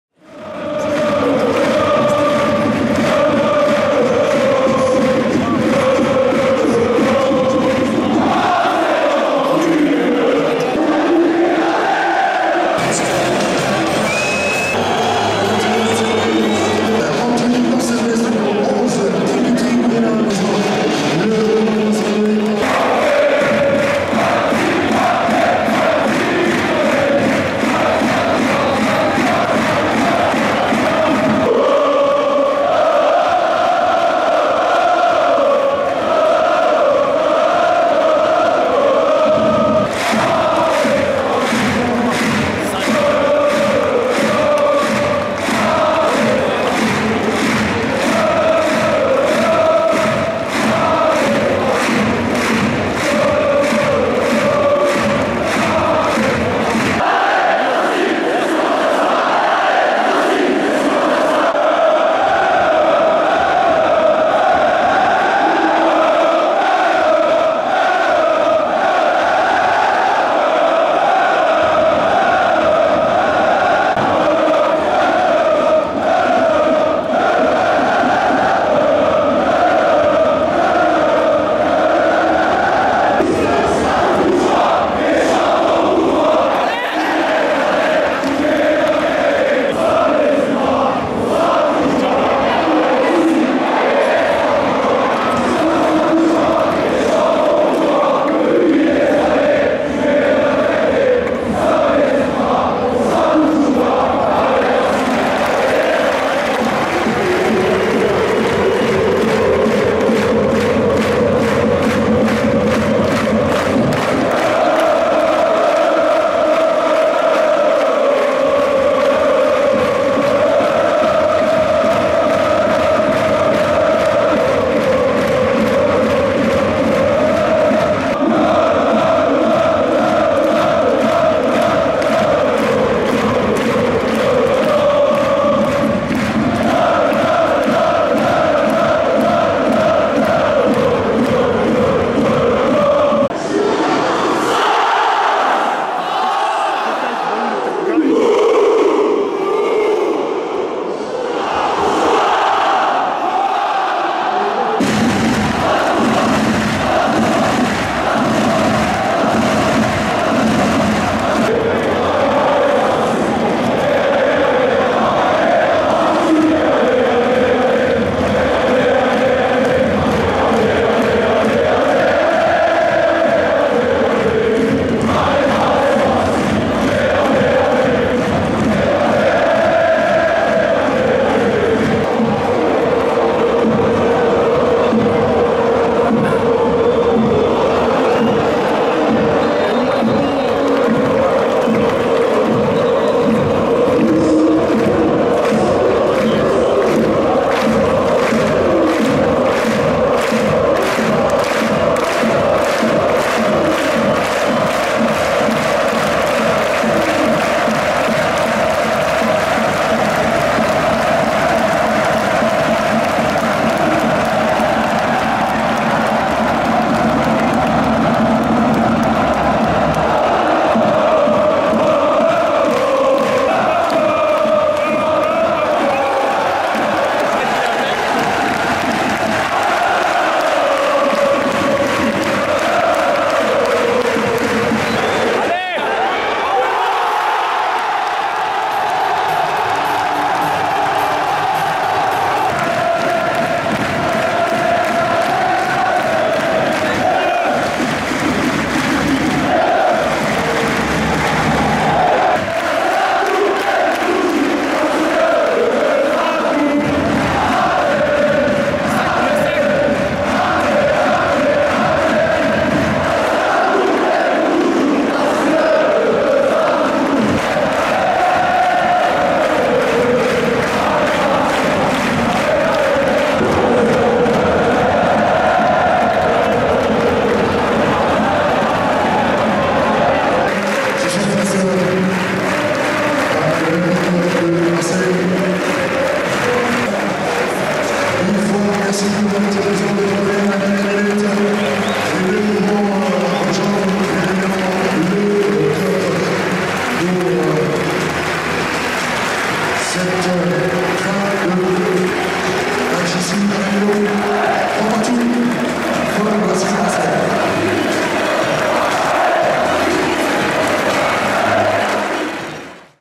RC STRASBOURG - OLYMPIQUE DE MARSEILLE : 3-3 (JOURNÉE 9 LIGUE 1 - 15/10/17)
Match de Ligue 1 opposant Strasbourg et Marseille lors de la 9ème journée (15/10/17) avec 26 500 spectateurs au stade de la Meinau ! Une ambiance exceptionnelle, pour un match exceptionnel ! Allez les bleus, allez racing !

(Toutes les images sont tournées par mes soins)